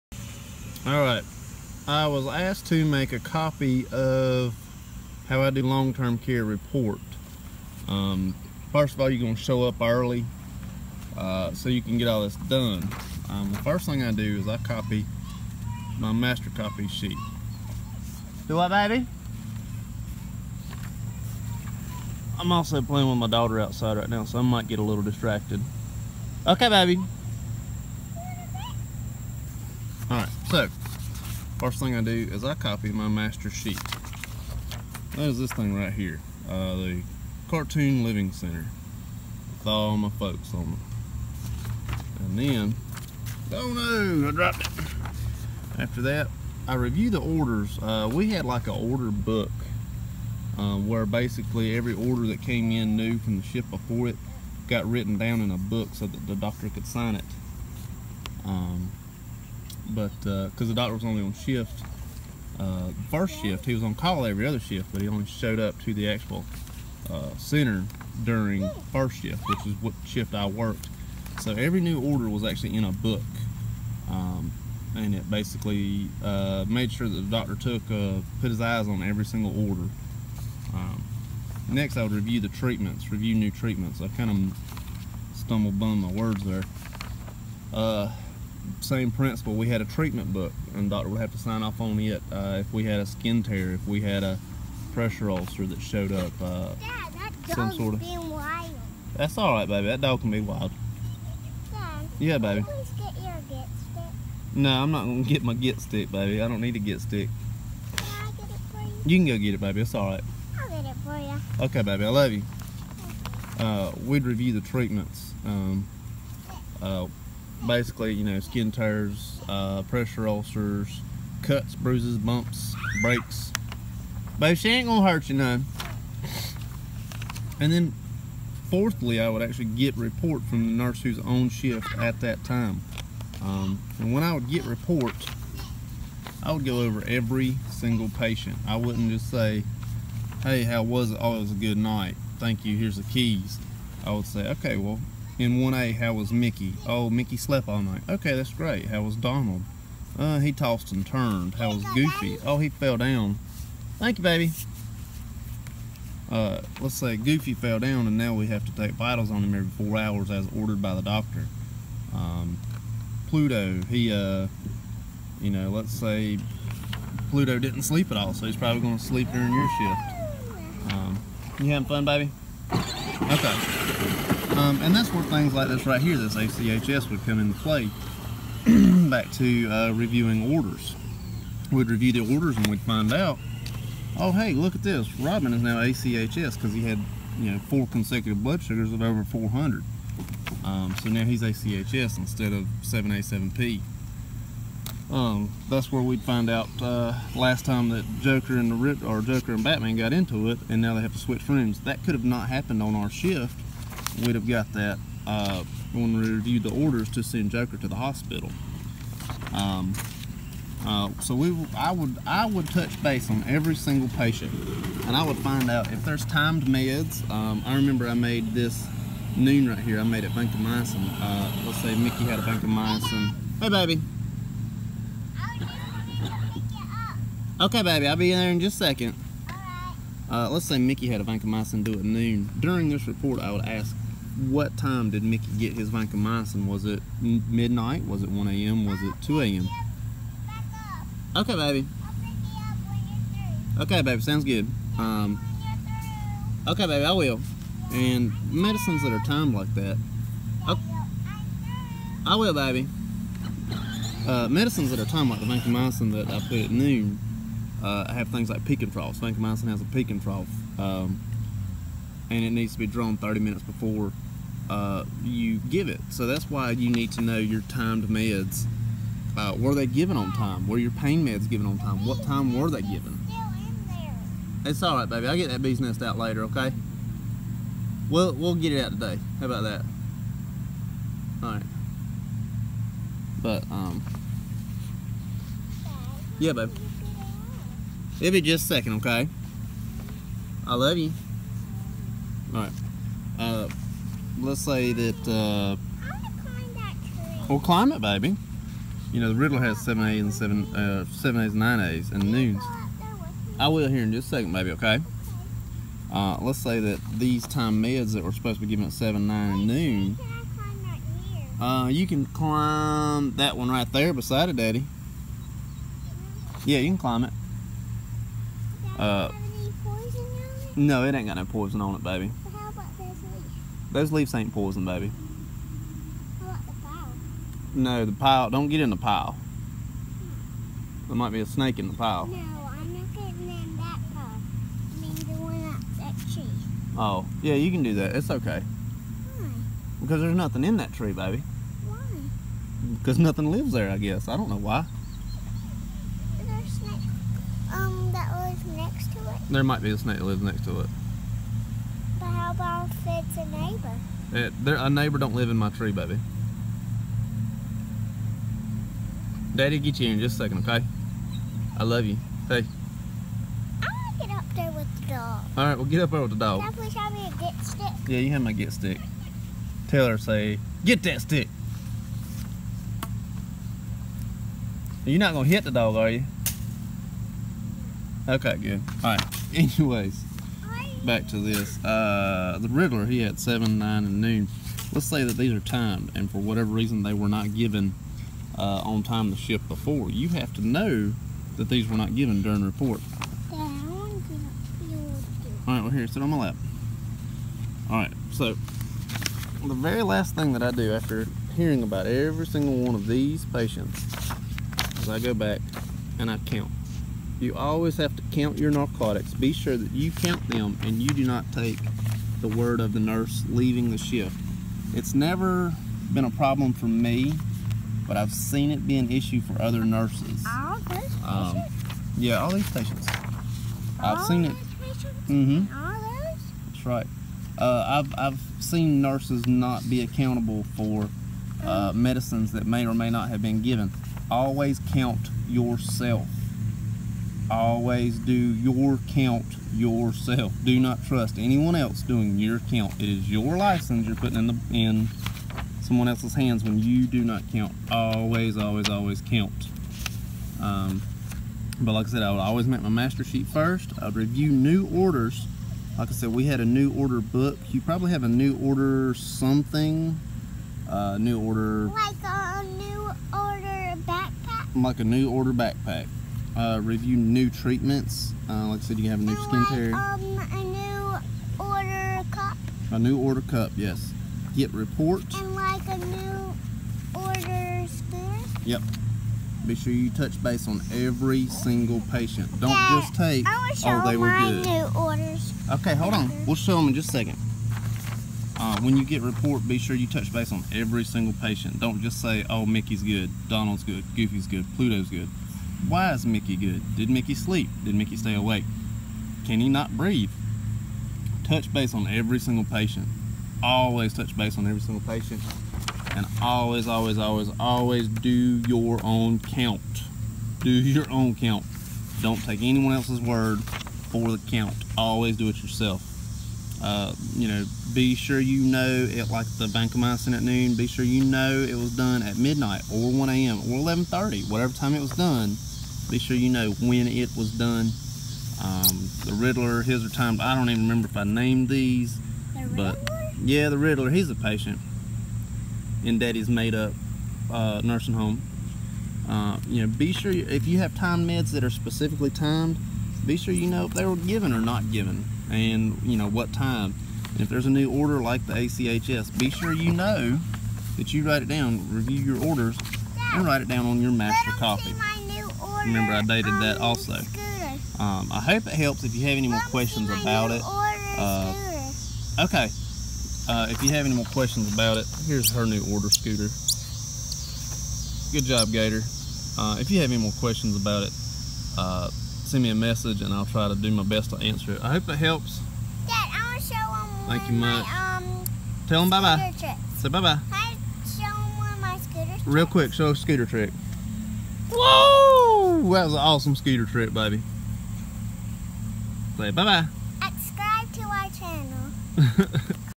Long term care report steps I used
1 copy master sheet
2 review orders
3 review treatment
4 get report on ALL patients
5 do your count, COUNT yourself also